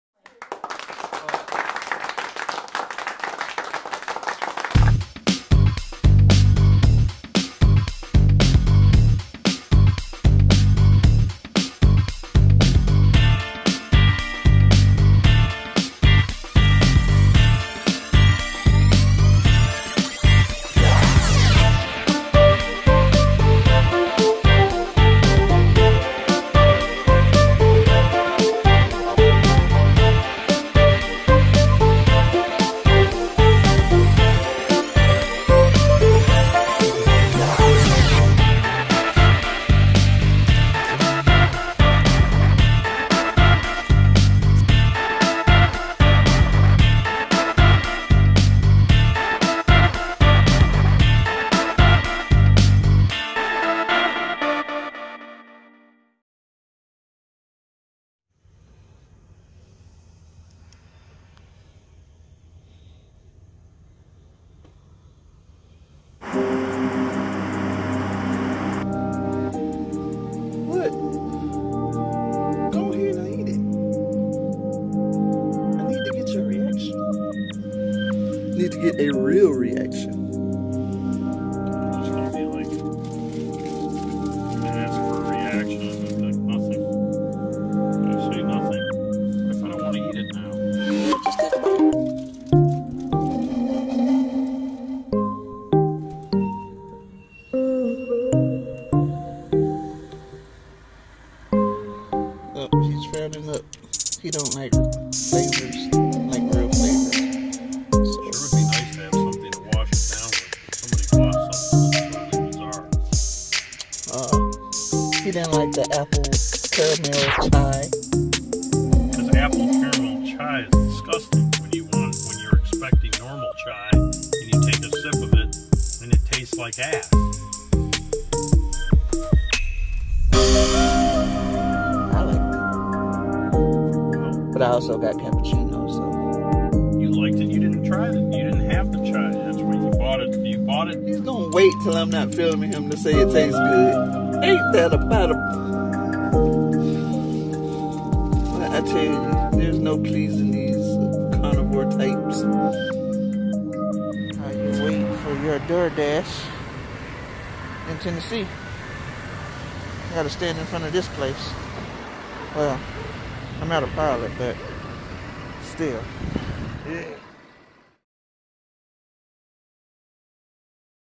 SleepyHead Coffee Review
A Stop in  downtown Chattanooga that didnt disappoint..for me anyway they have an assortment of vegan pastries and all drinks can be vegan!

camera used: Note 8

Edited: kinemaster